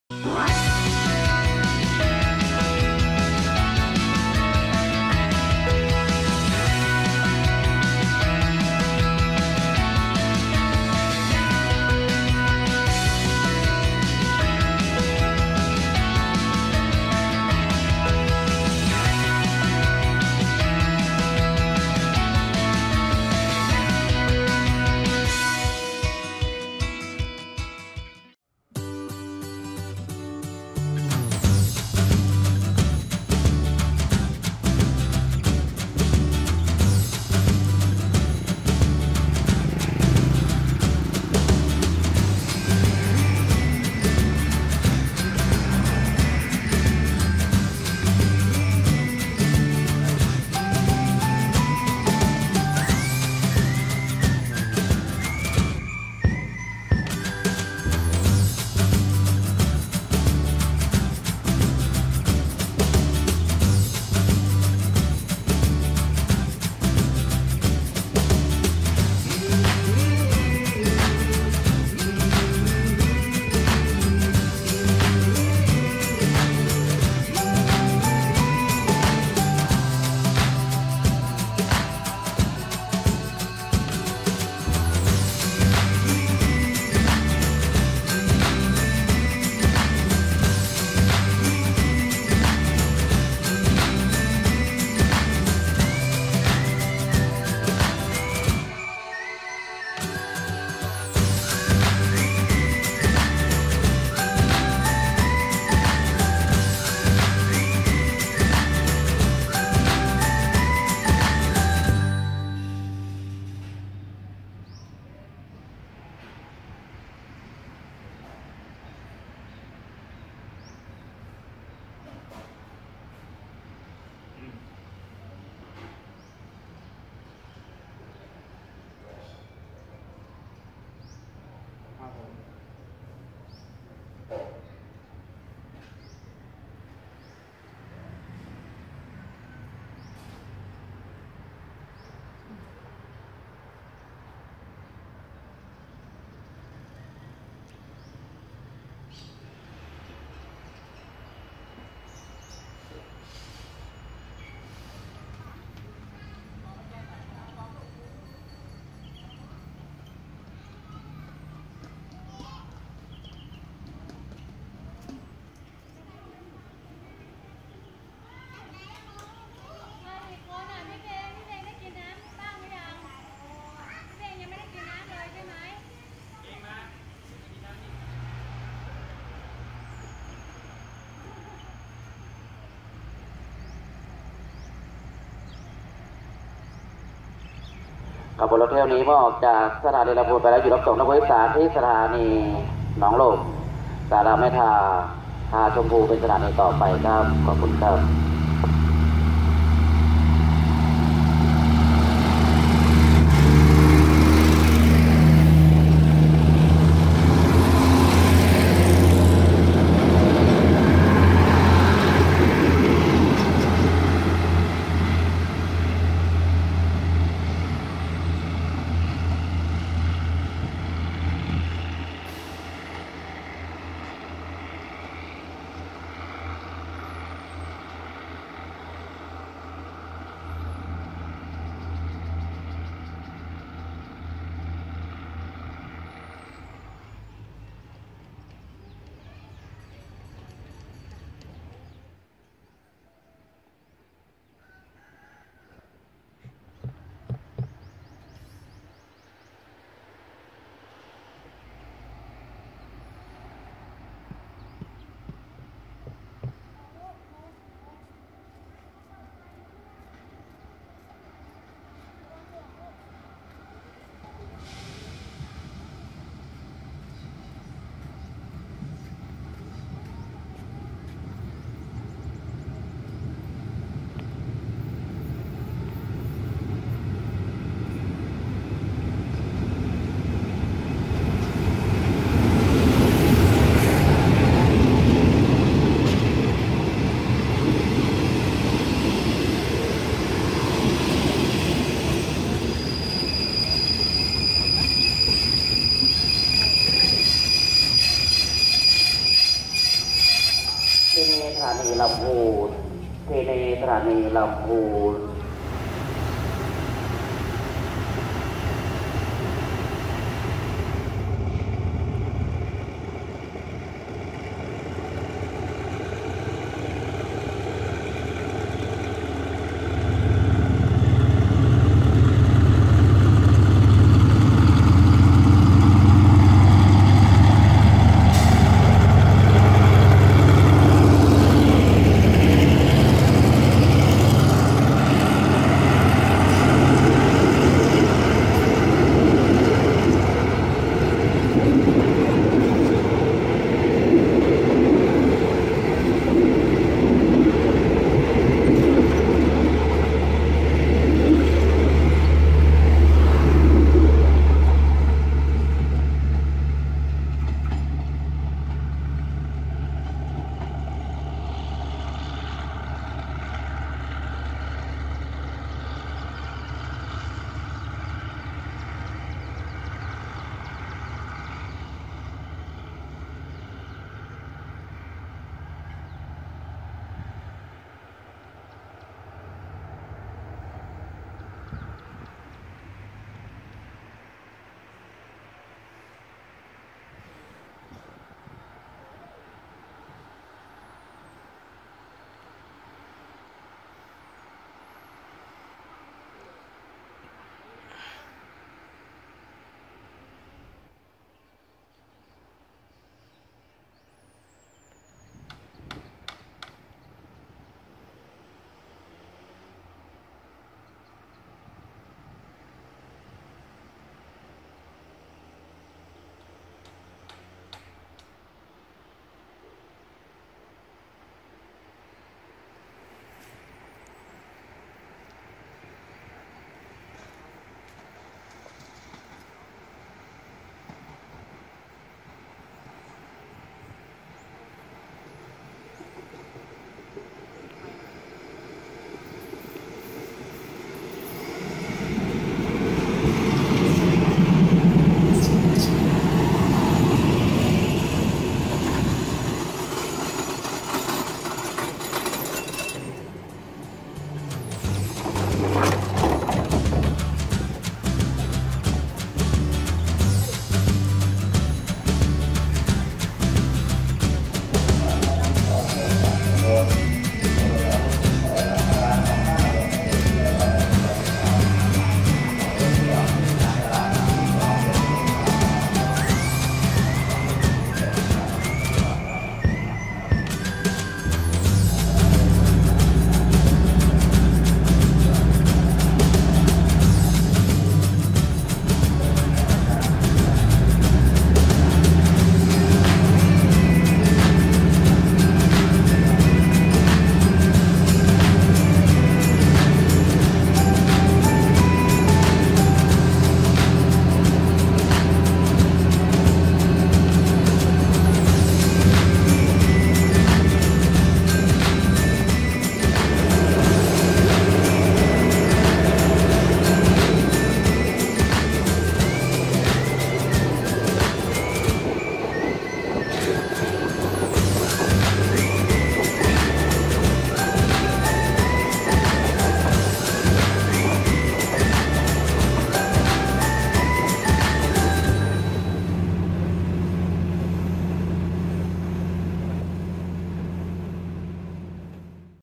Thai Railway: Train arrivals and departures at Lamphun Station
Lamphun Railway Station
Lamphun, Thailand

Northern Line, 729 km from Bangkok
Operating by State Railway of Thailand

THAIRAIL CHANNEL